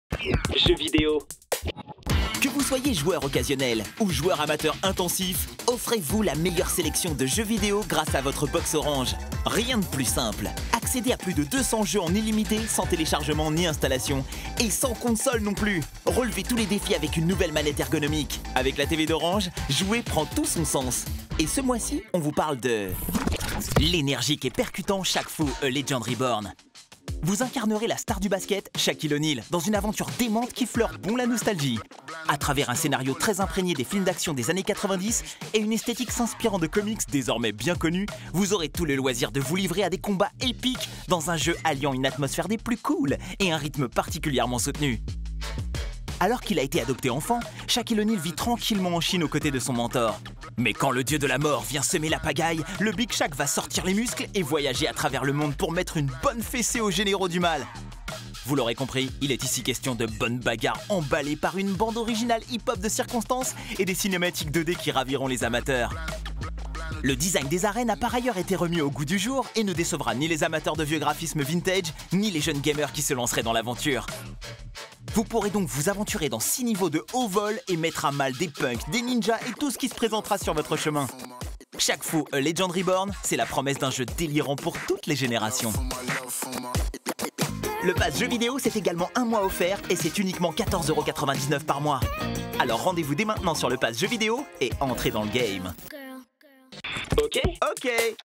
Actu Pass Jeux Vidéo septembre 2020
Ce mois-ci, on vous présente le jeu Shaq Fu.